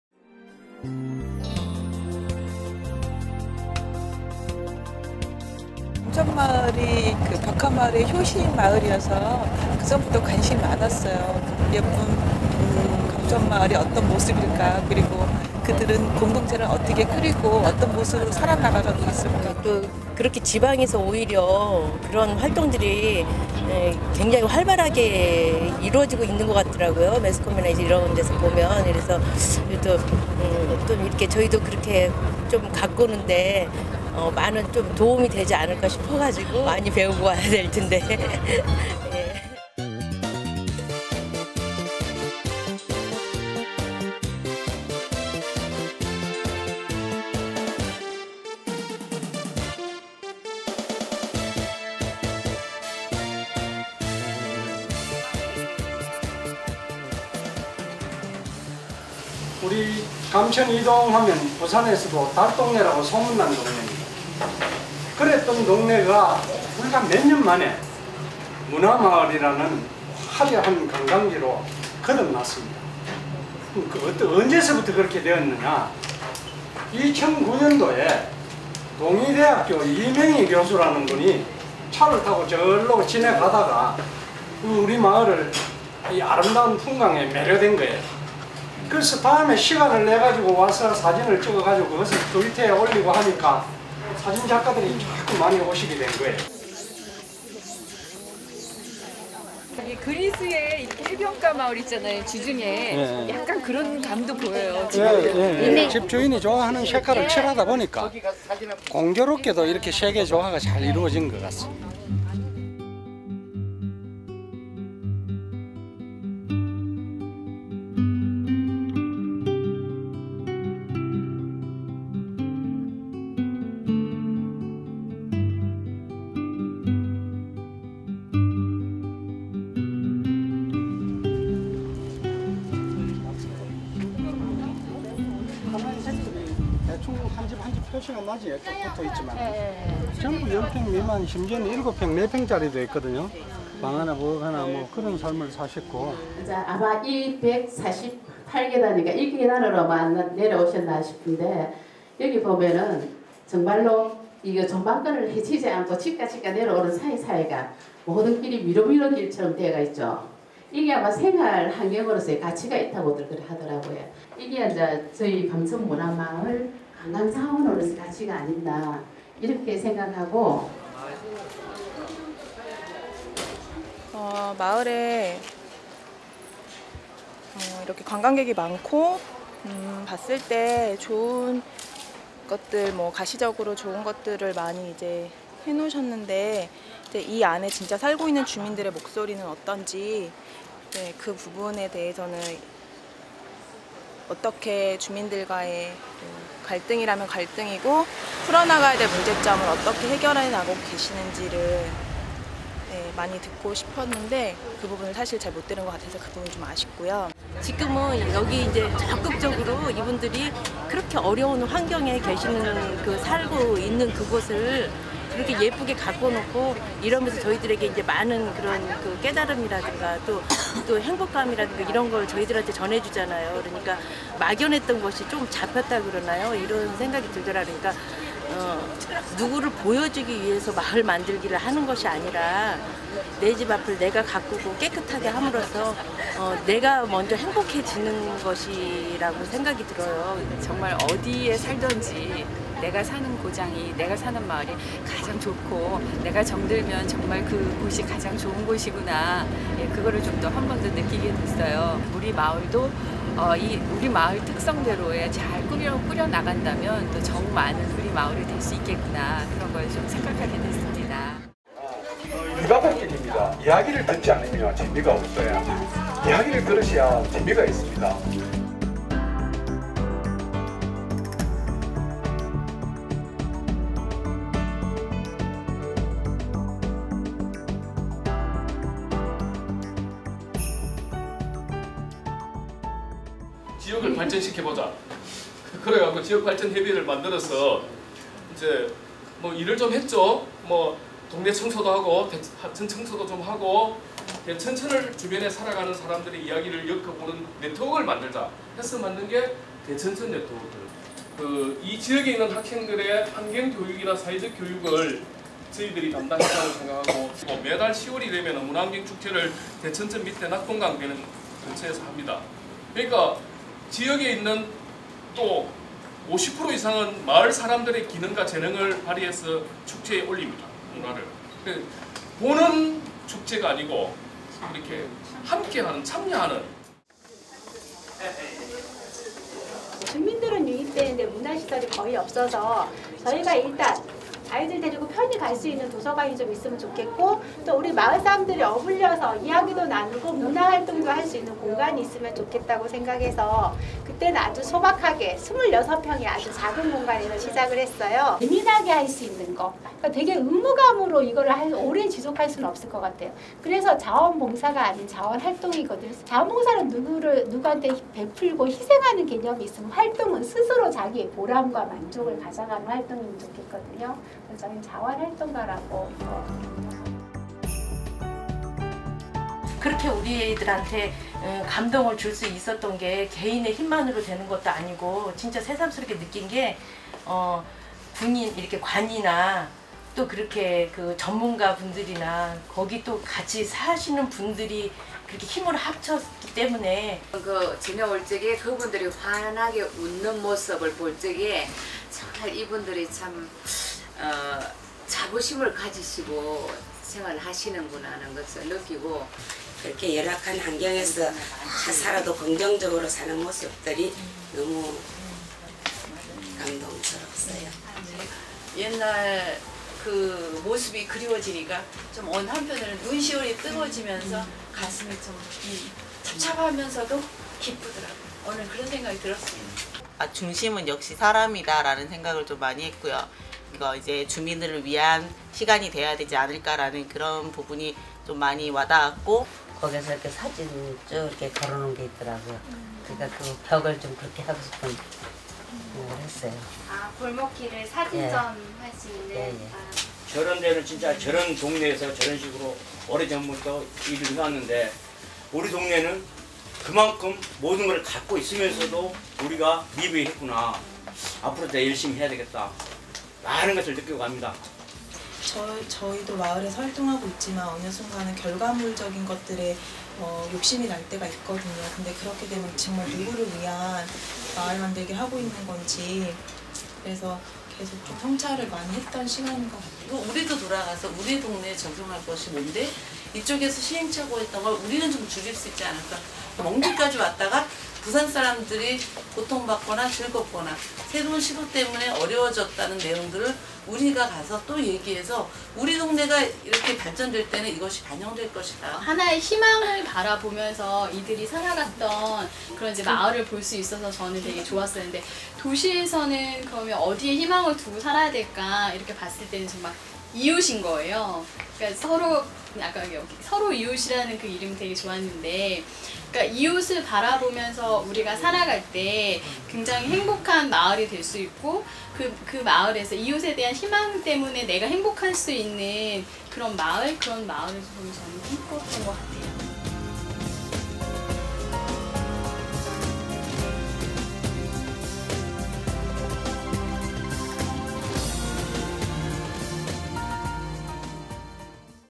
[마을포커스10] 8월.. 성북주민들, 부산으로 마을견학 고고~
매월 1가지 주제로 찾아뵙는 마을포커스!
8월에는 부산 감천마을로 함께 견학을 떠난 성북주민들의 여정을 카메라에 담았습니다
어떤 풍경, 어떤 배움, 어떤 이야기들이 쏟아져 나왔을까요?